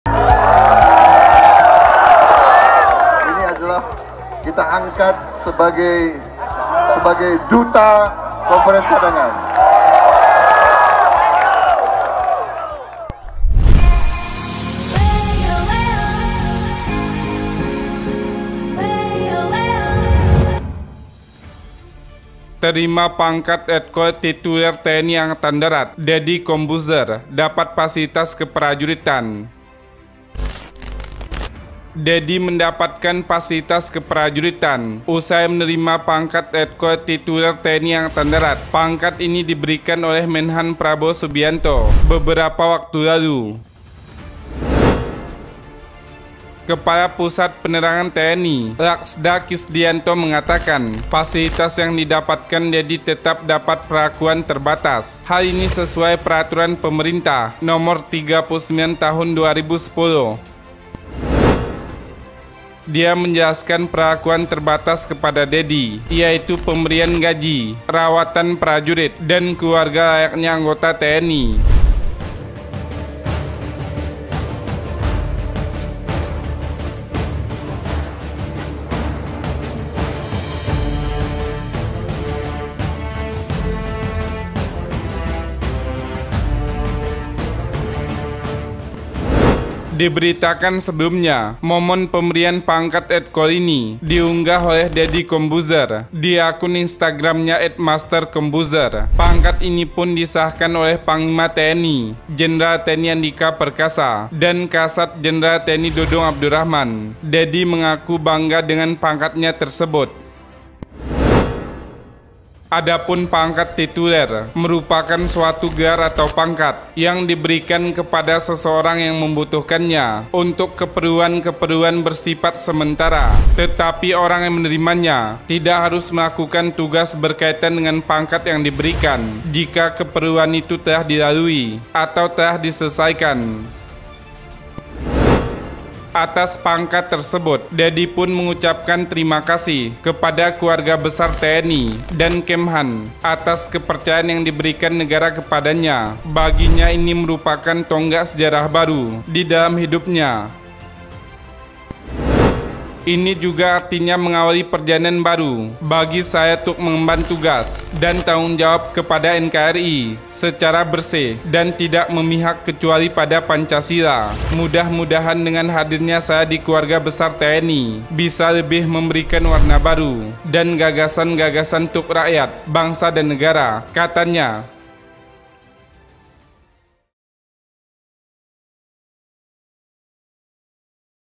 Deddy Corbuzier Terima Pangkat Letkol Tituler TNI AD dan Jadi Prajurit TNI
Terima Pangkat Letkol Tituler TNI AD, Deddy Corbuzier Dapat Fasilitas Keprajuritan

Deddy mendapatkan fasilitas keprajuritan usai menerima pangkat (Letkol) Tituler TNI Angkatan Darat. Pangkat ini diberikan oleh Menhan Prabowo Subianto beberapa waktu lalu. Kepala Pusat Penerangan (Kapuspen) TNI Laksda Kisdiyanto mengatakan fasilitas yang didapatkan Deddy tetap dapat perlakuan terbatas. Hal ini sesuai Peraturan Pemerintah Nomor 39 Tahun 2010

Dia menjelaskan perlakuan terbatas kepada Deddy yaitu pemberian gaji, rawatan prajurit, dan keluarga layaknya anggota TNI.

Diberitakan sebelumnya, momen pemberian pangkat Letkol ini diunggah oleh Deddy Corbuzier di akun Instagramnya @mastercorbuzier. Pangkat ini pun disahkan oleh Panglima TNI Jenderal TNI Andika Perkasa dan KSAD Jenderal TNI Dudung Abdurachman. Deddy mengaku bangga dengan pangkatnya tersebut.

Adapun, pangkat Tituler merupakan suatu gelar atau pangkat yang diberikan kepada seseorang yang membutuhkannya untuk keperluan-keperluan bersifat sementara, tetapi orang yang menerimanya tidak harus melakukan tugas yang berkaitan dengan gelar/pangkat yang diberikan jika keperluan itu telah dilalui atau telah diselesaikan. Atas pangkat tersebut, Deddy pun mengucapkan terima kasih kepada keluarga besar TNI dan Kemhan atas kepercayan yang diberikan negara kepadanya. Baginya, ini merupakan tonggak sejarah baru di dalam hidupnya.

"Ini juga artinya mengawali perjalanan baru bagi saya tuk mengemban tugas dan tanggung jawab pada NKRI secara bersih dan tidak memihak kecuali pada Pancasila. Mudah mudahan dengan hadirnya saya di keluarga besar TNI bisa lebih memberikan warna baru dan gagasan-gagasan tuk Rakyat, Bangsa, dan Negara," ujarnya.